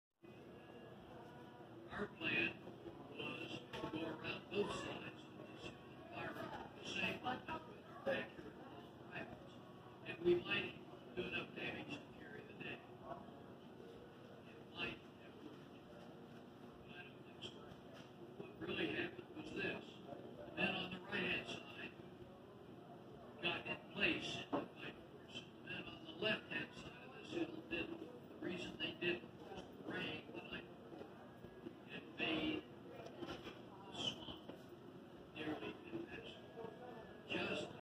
Bee Dancing to Show the Hive Where to Find Food
We took a drive in the mountains and at one stop, they had this beehive on display inside the building. The hive is completely sealed behind the plexiglass with a pipe allowing the bees to fly in and out from outside. This bee was showing the rest of the hive where to go to find food. It's amazing to read about, but even more so to witness firsthand. They've had this hive for a long time and it's always a joy to watch!